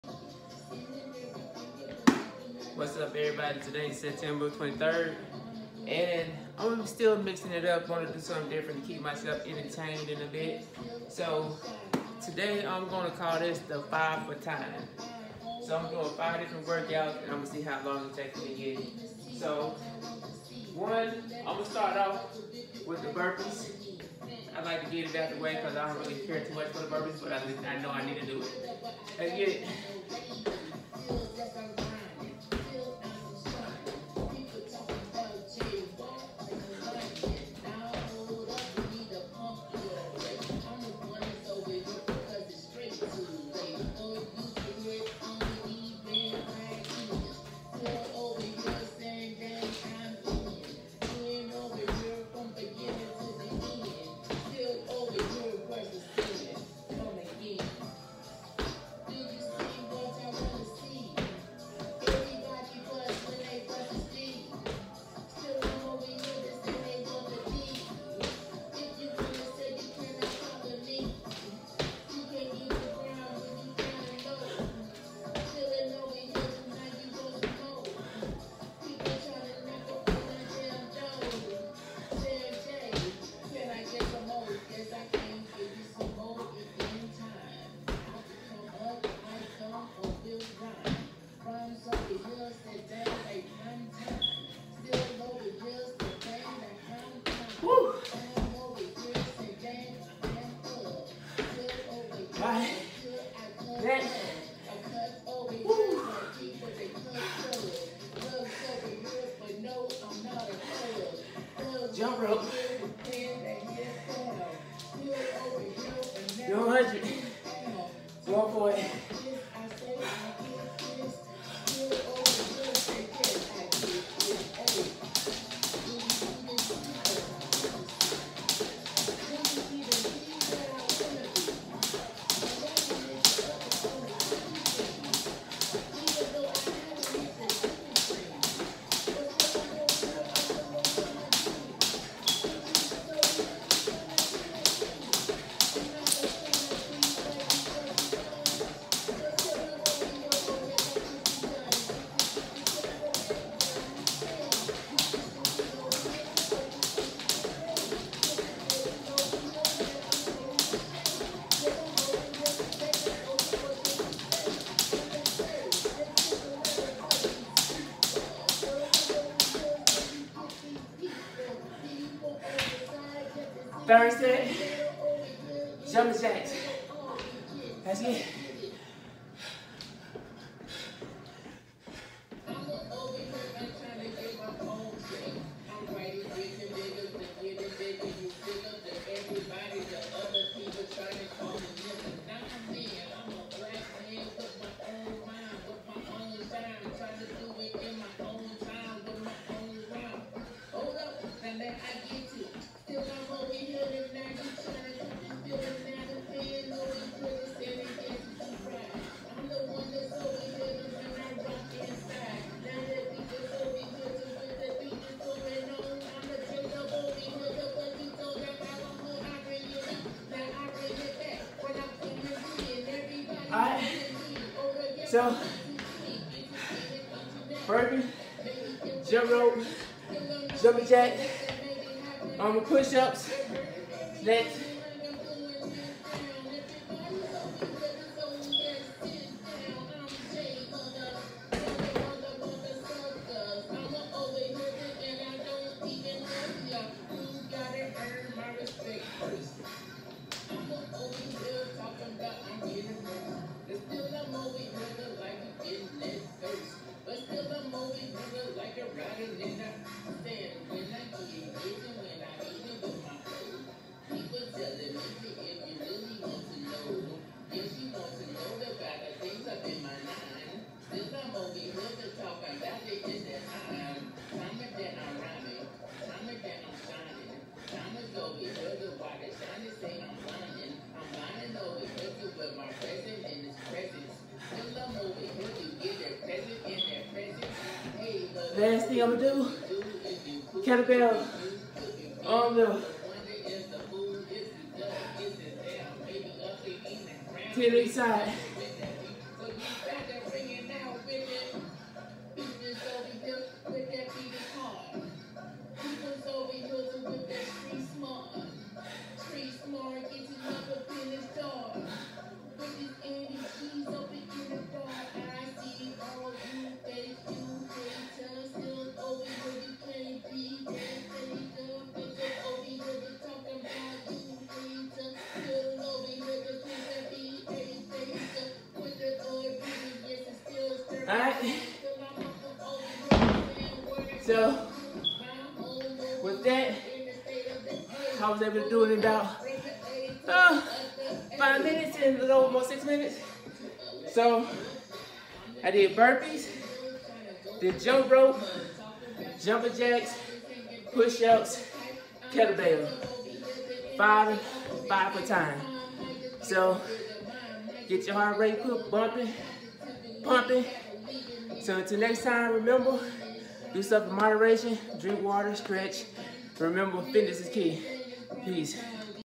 5 workouts for time. ThejJordanFitness FitnessIsKey🔑
Today’s workout
1. 20 burpee
2. 100 jump rope
3. 50 jumping jacks
4. 40 pushups
5 10 rotation arm kettle bell
Time under 6 minutes